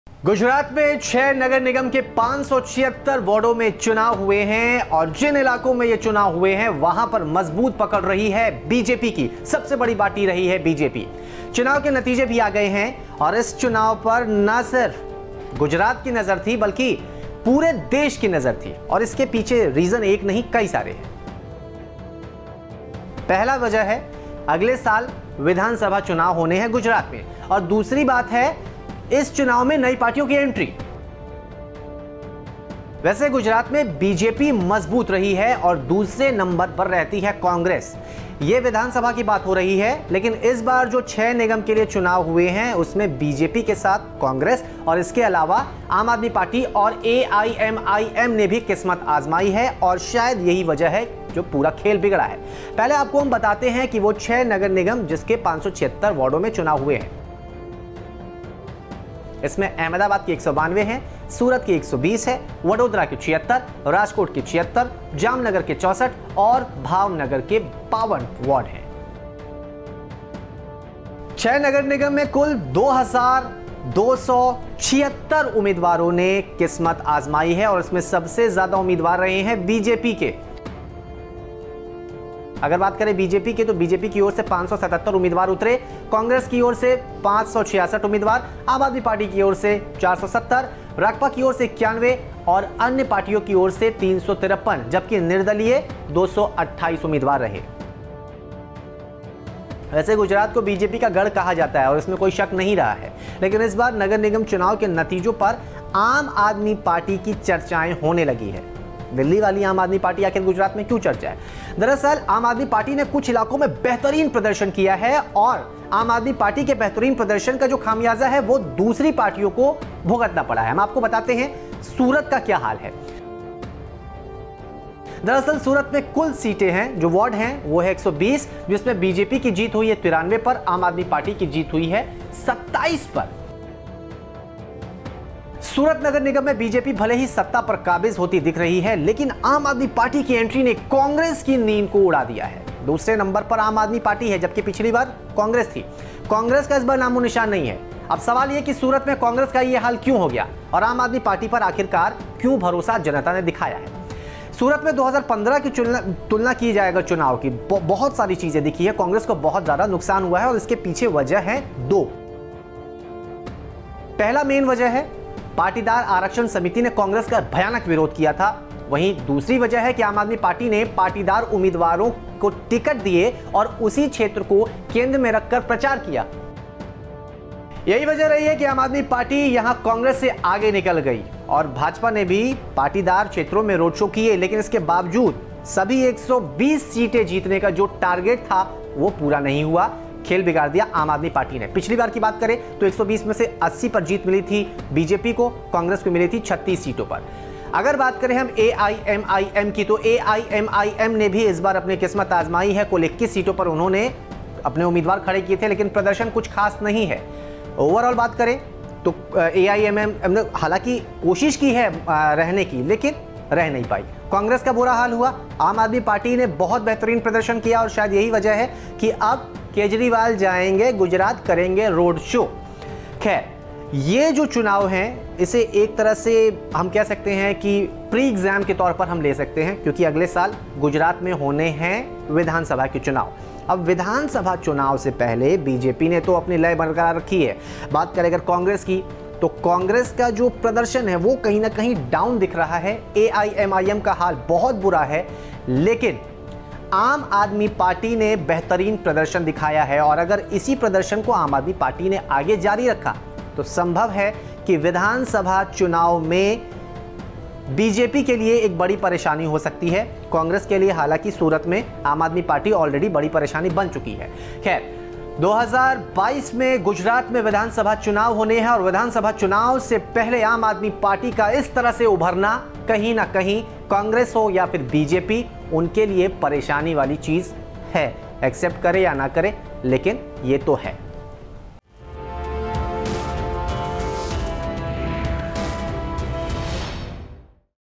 Gujarat Municipal Election Results: क्या है वो वजह जिसके कारण AAP को हुआ फायदा ?
-  गुजरात में 6 नगर निगम के 576 वार्डों में चुनाव हुए... जिन इलाकों में चुनाव हुए हैं वहां पर सबसे ज्यादा पकड़ बीजेपी की रही है... चुनाव के नतीजे भी आ गए हैं और इस चुनाव पर पर ना सिर्फ गुजरात की नजर थी बल्कि पूरे देश की नजर थी.. इसके पीछे वजह एक नहीं कई सारे हैं...